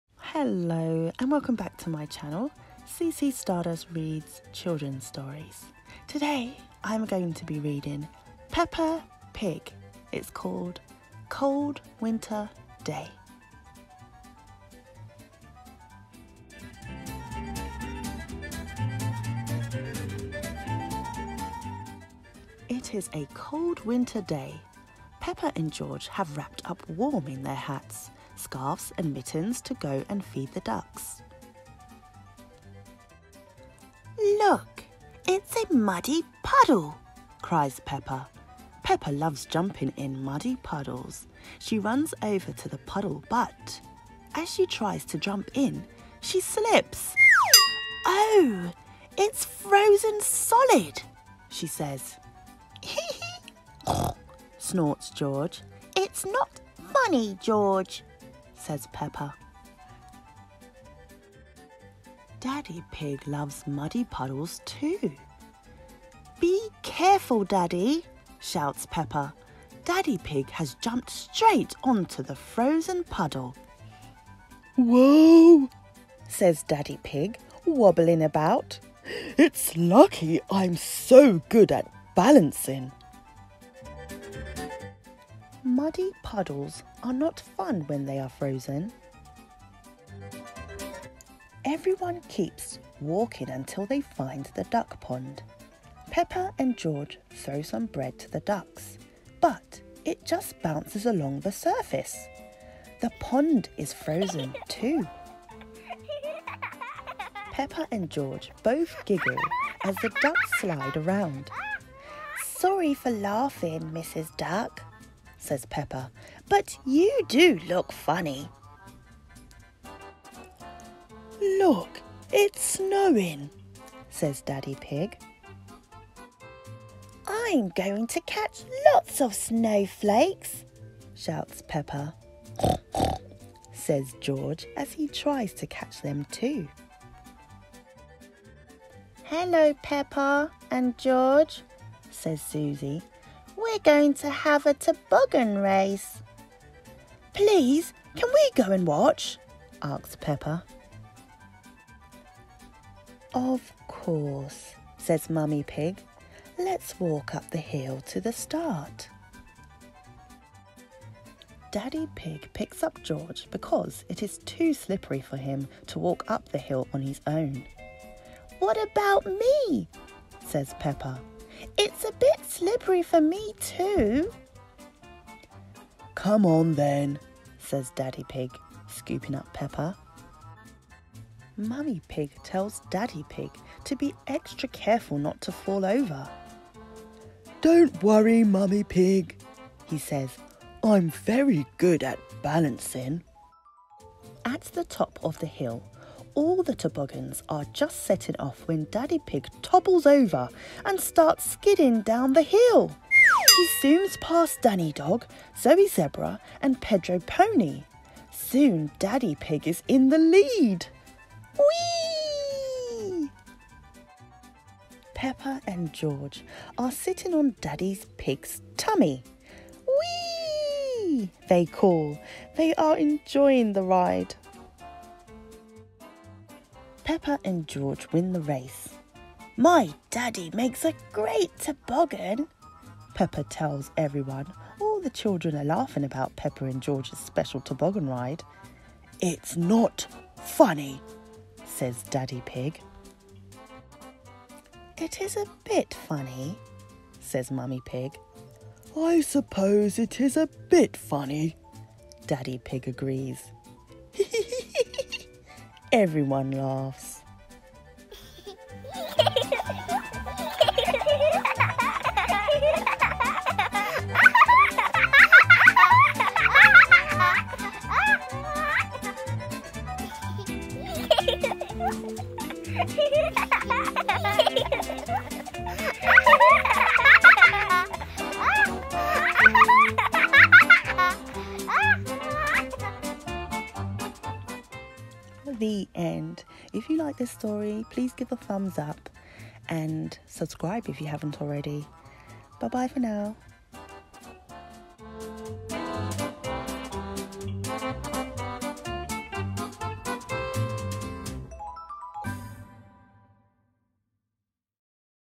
Peppa Pig Cold Winter Day | children’s story time | Read aloud by CC Stardust
Cold Winter Day Peppa Pig reading book
“It is a Cold winter day and all the puddles have frozen over! Jumping in icy puddles it’s not fun￼, But there are lots of other exciting things to do in the snow￼! “￼